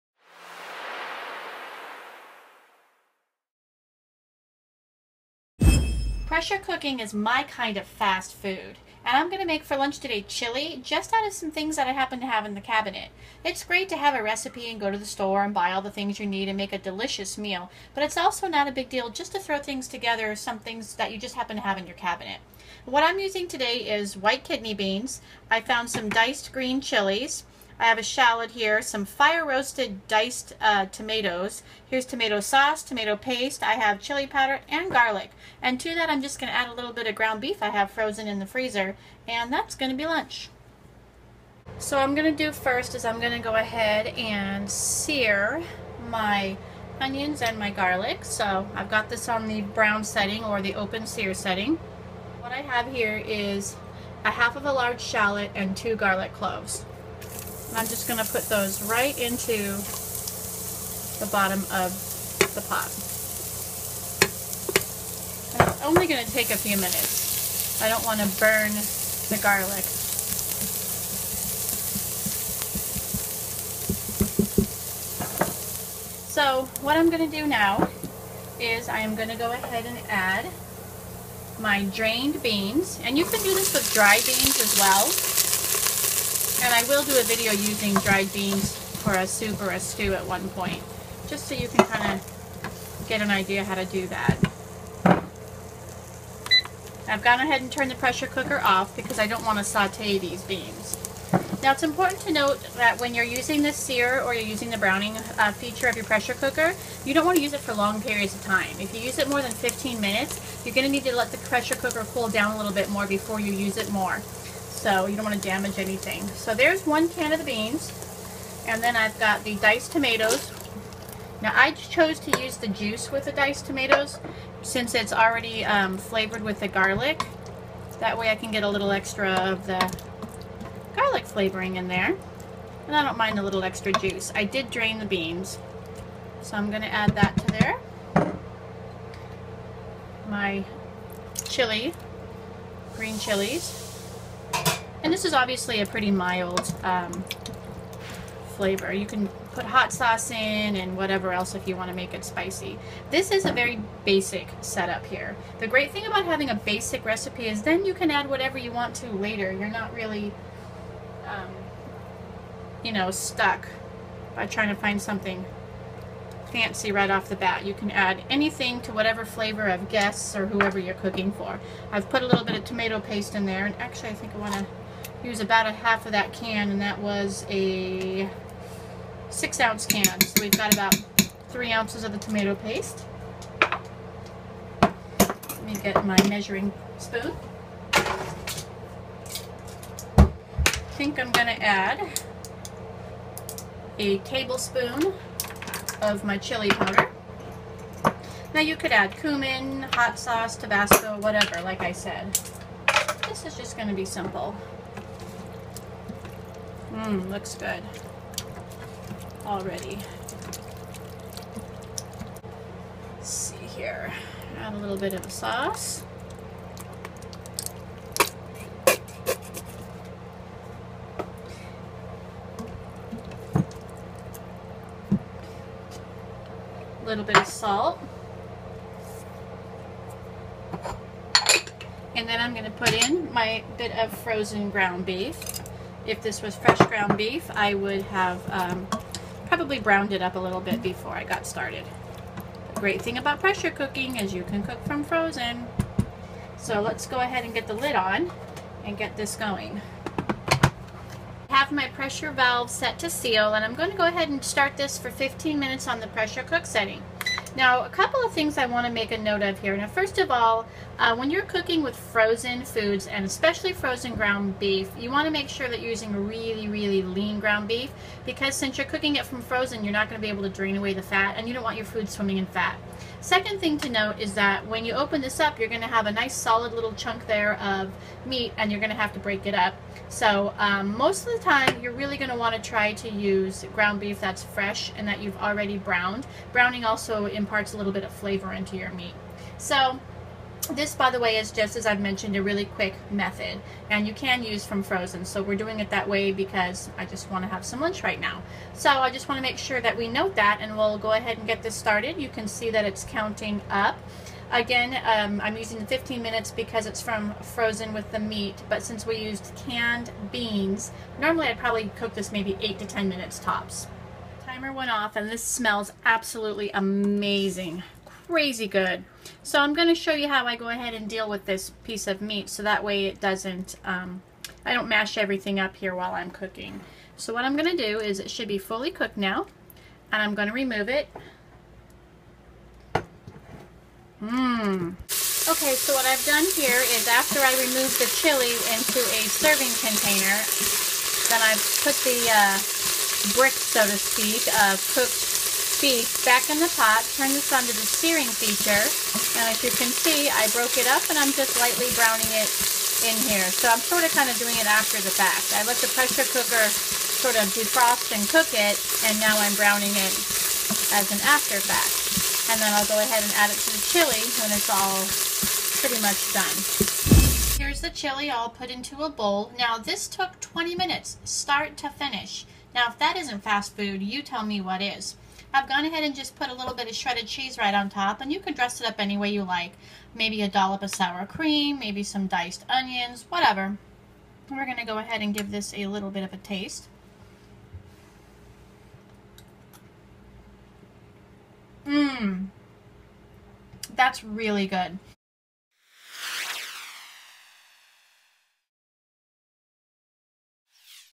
20 Minute Chili (Pressure Cooker)
This is a basic no frills chili recipe cooked in only 20 minutes in the pressure cooker. Add your own touches to make a delicious chili or use a favorite recipe of your own.